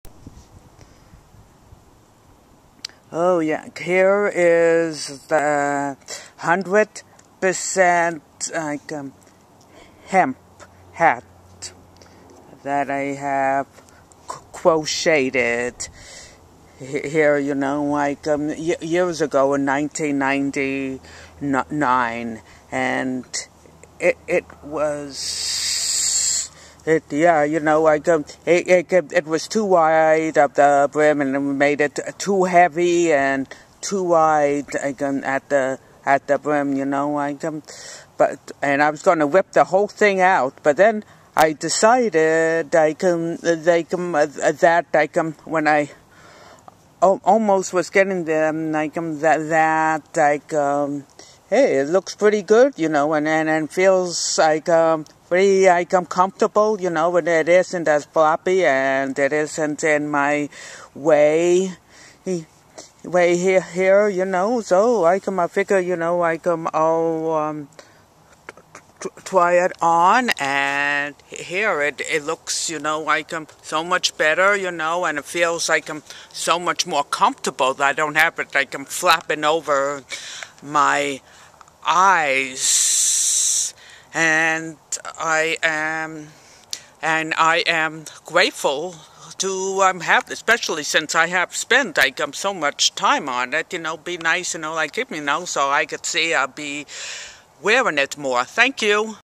Hand Crochet Hemp Hat (Revised)
I was going to unravel all of this hemp hat until I tried it on after about 6 centimeters of the rim was removed.  It felt so much lighter and more comfortable with 5 centimeters of the rim left.  It did not get in the way of my eyes since it was not so flappy anymore.  I am keeping and wearing this hat.